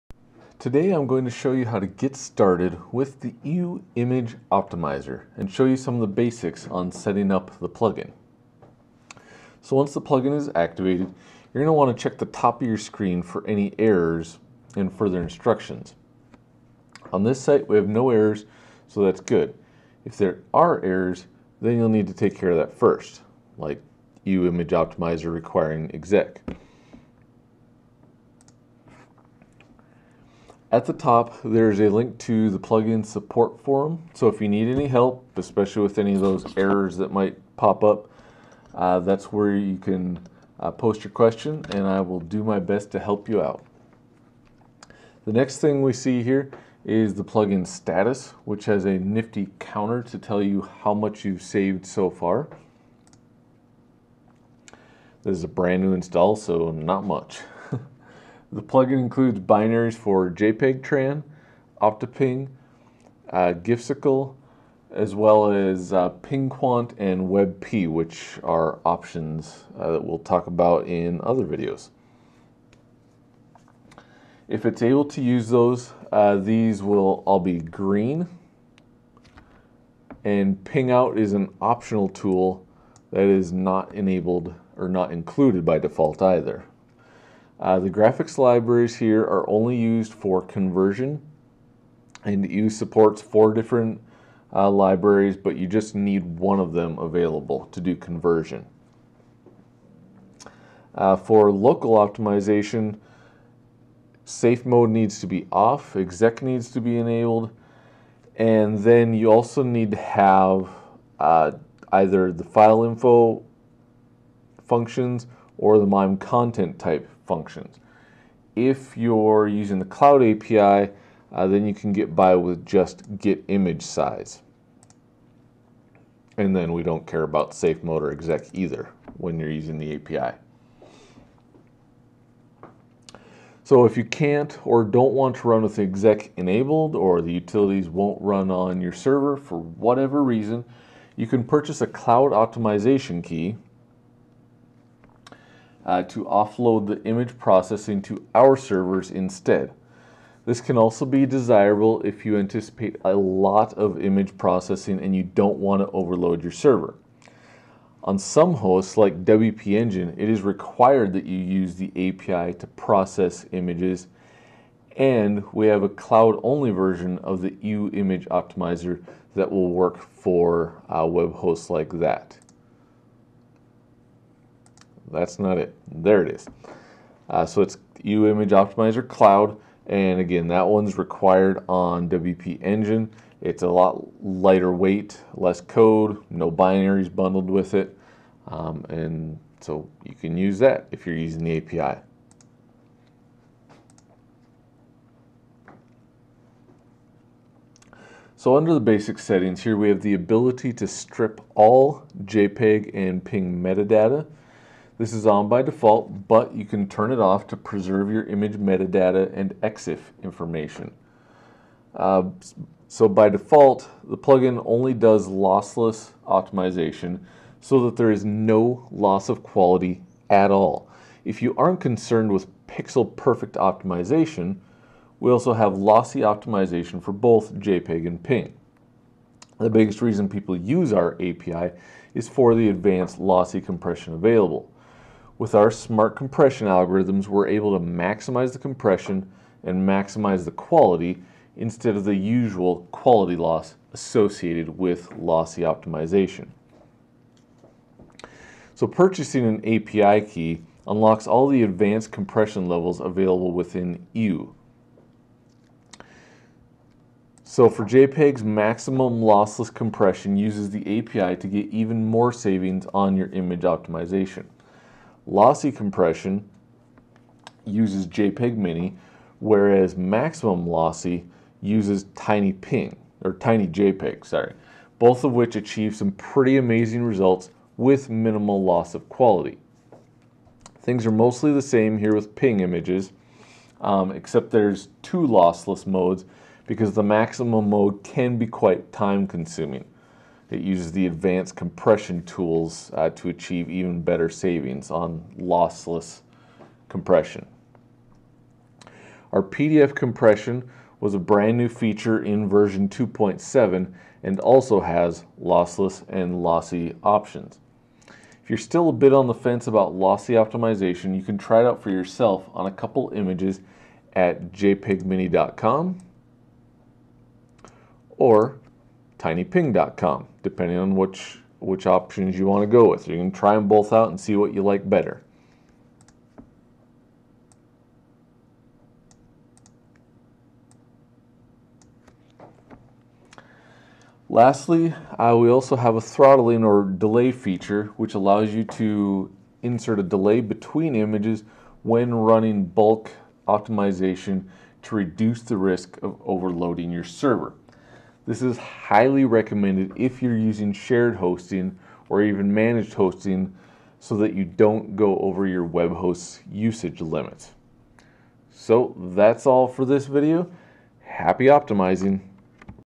EWWW Image Optimizer - Getting Started
We show you the basics of setting up the plugin for Wordpress